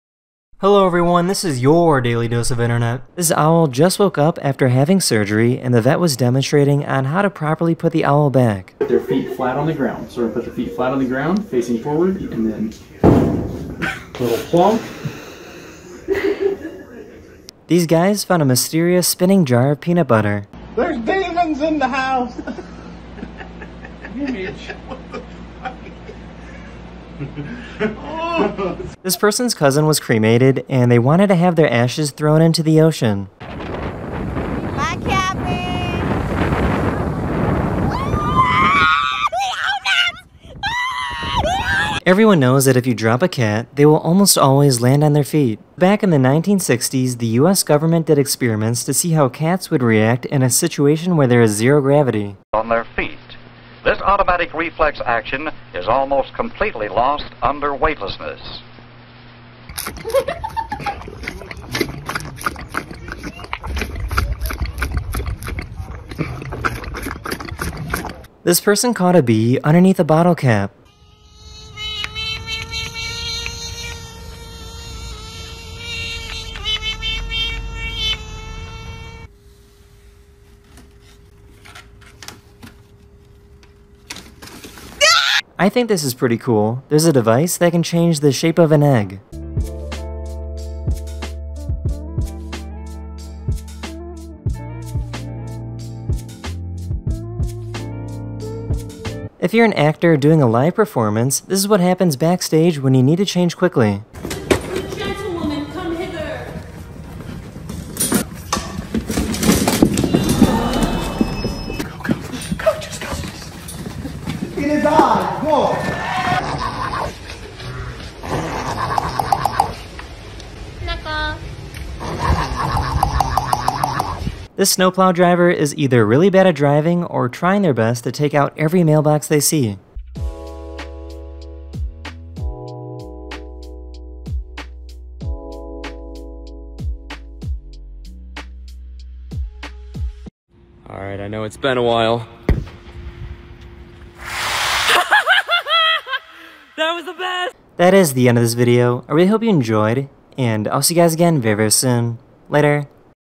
The Echo Shook the Pool
Hello everyone, this is YOUR Daily Dose of Internet. In this video, I show you how someone does a cool trick with water at an indoor pool area.

Also in this video, I show you a device that can change the shape of an egg. A broken owl A seal makes a very strange noise. A snowplow runs over mailboxes.